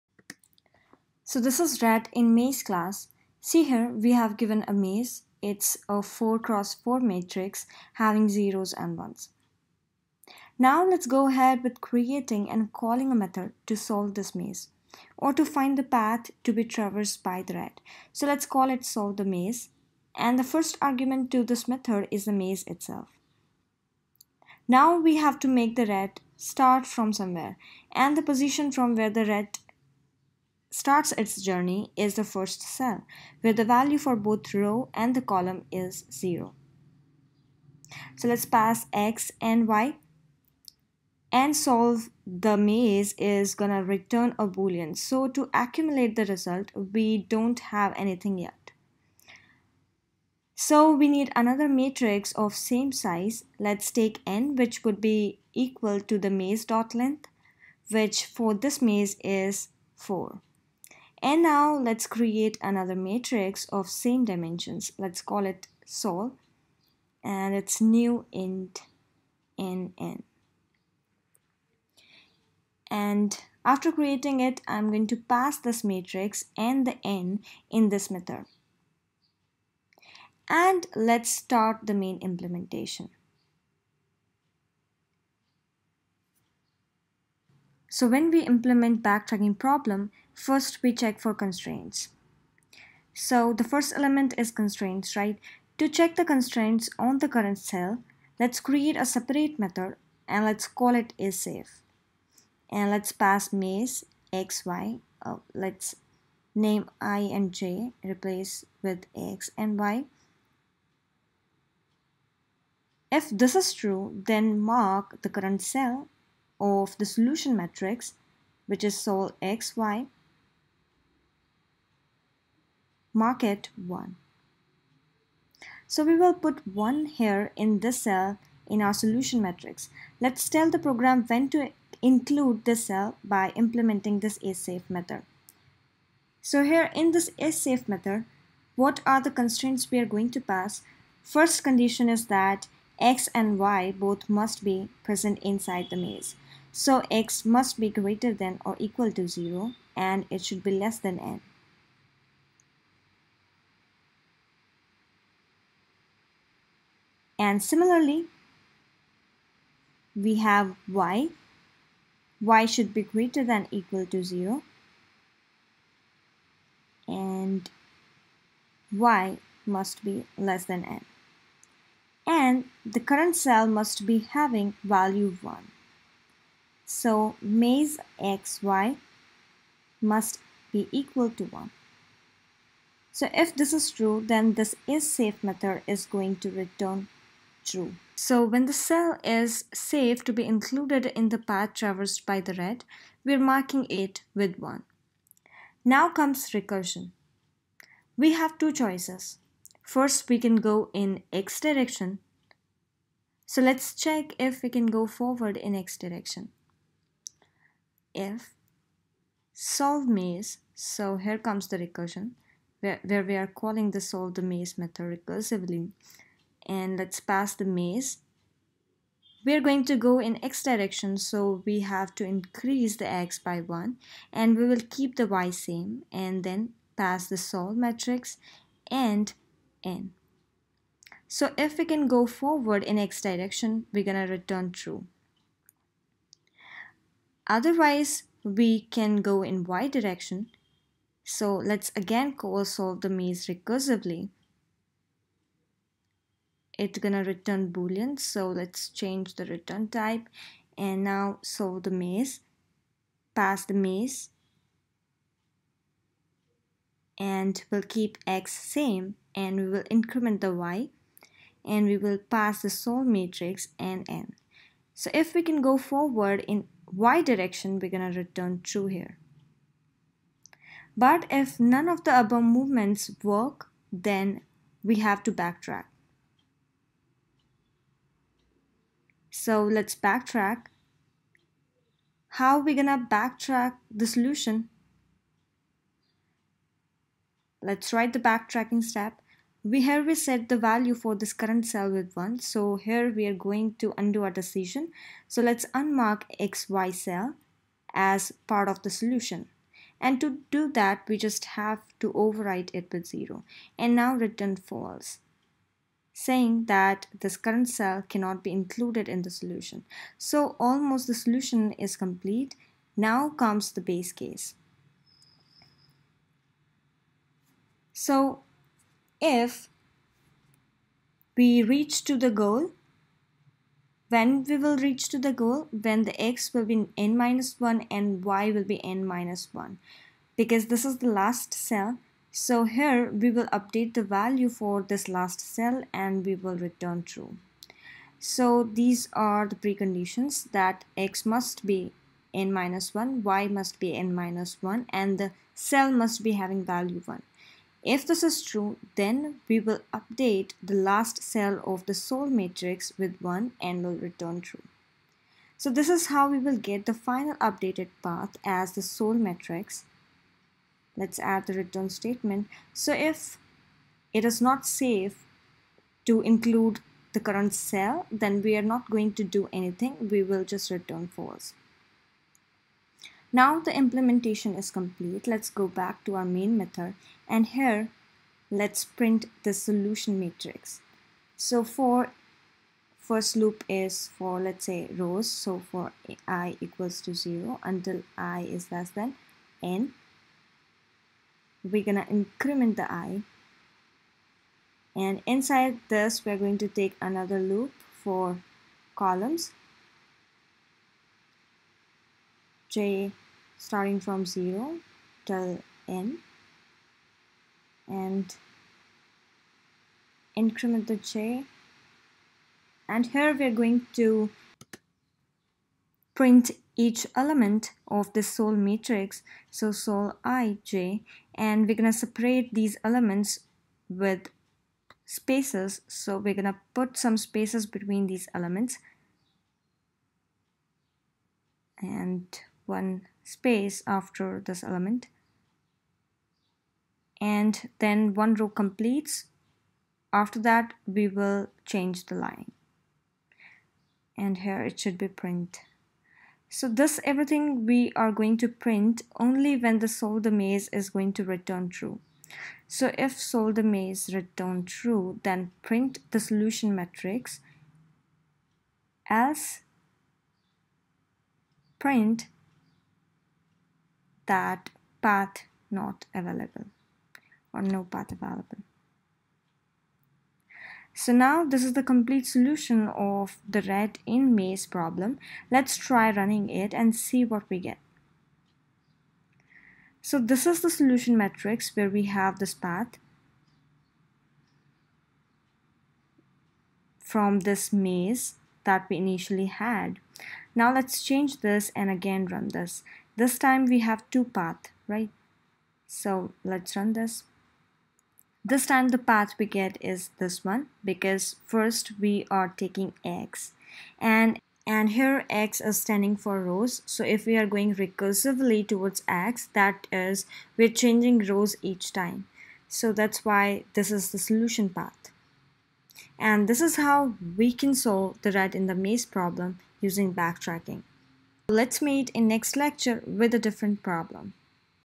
🐭 Solve the Rat in a Maze Problem | Backtracking Algorithm Explained Step-by-Step!" PART-2
In A Maze is the backtracking problem we gonna solve in this video. Here is the complete code solution for this backtracking problem.

So, basically, you are given a maze, a rat starts from the top left corner and has to reach the destination which is the lower right-most corner to get out of the maze.
There are some obstacles or dead-ends in this maze, the rat can not cross these.

So, the rat can go forward in the x-direction or it can go down.

In the real problem, you'll be given a matrix having 0's and 1. 0 mean dead-end and 1 is the valid path to be traversed by the rat.